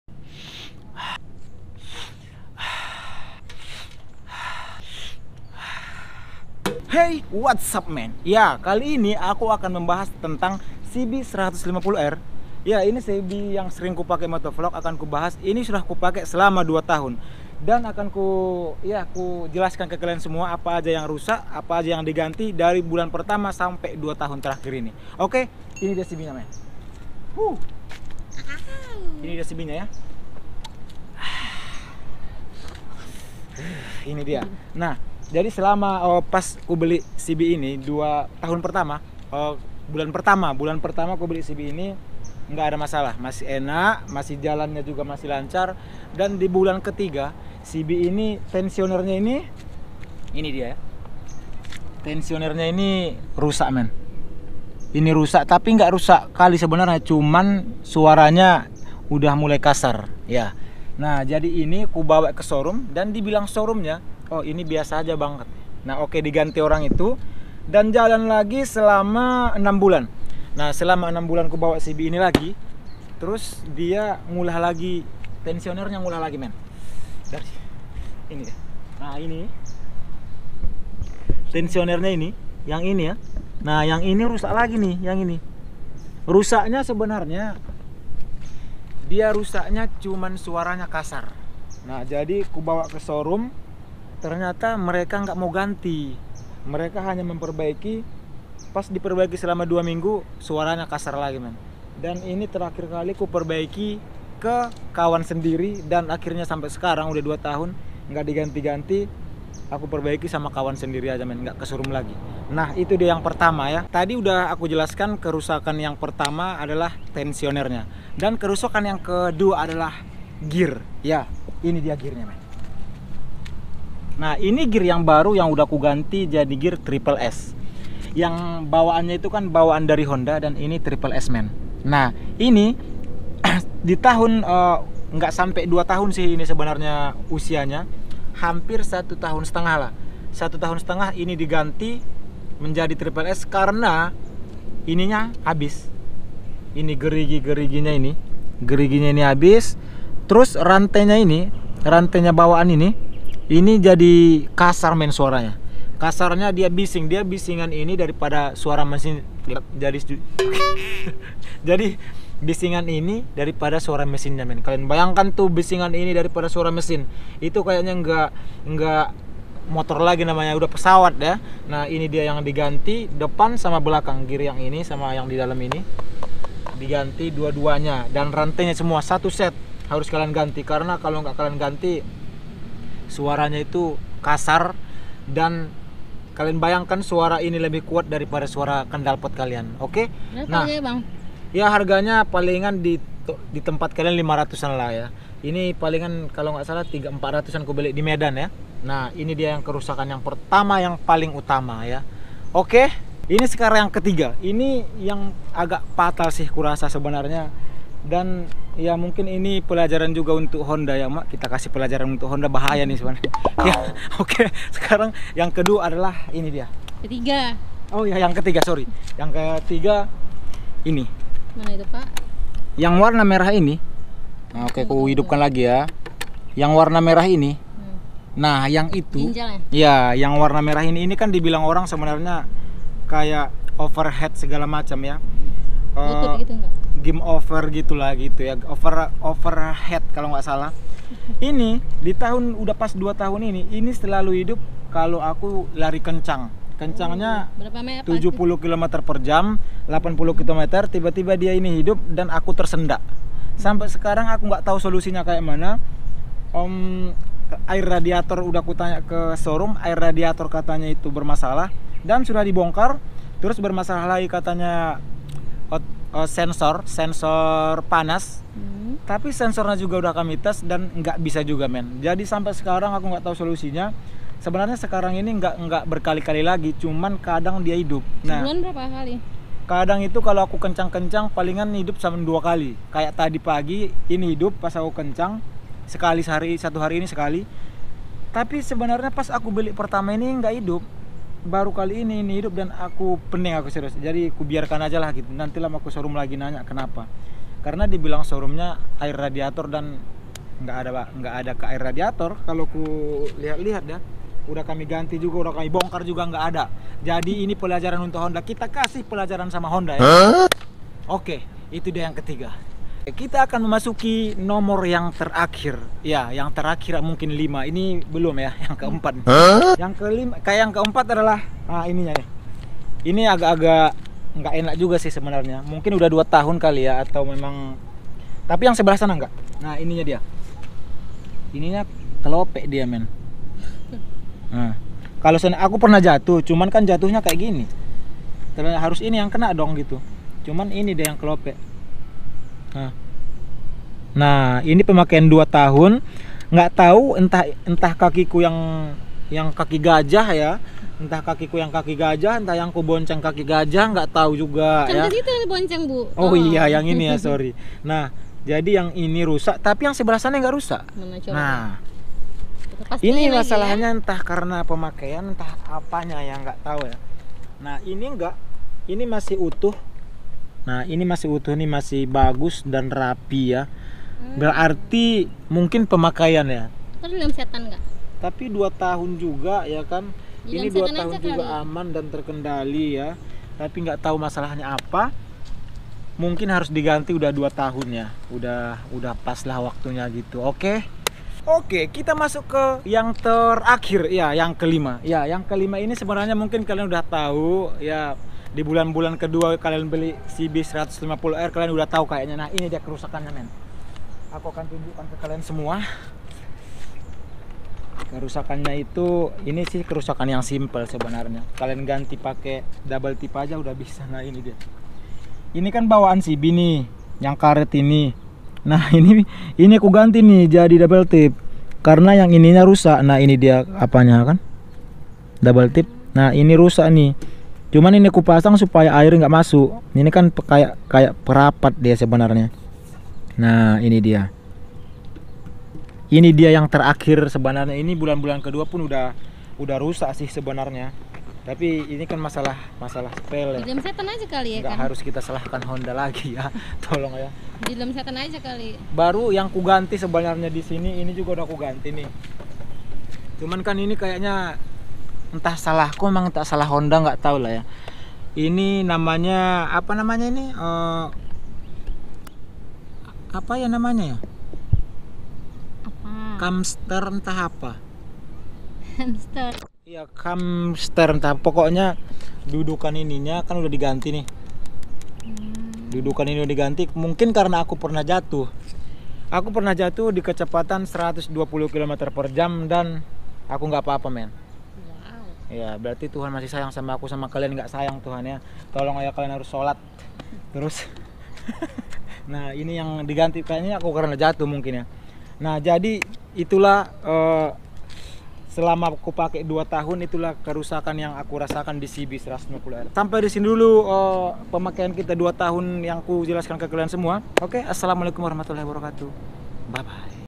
KERUSAKAN CB150R SELAMA PEKAMAKAIN 2 TAHUN
Cb150r yang aku pakai udah lebih 2 tahun, dan klo menurutku kerusakan cb150r gak ke mesin sih, hanya aksesoris aja yang rusak.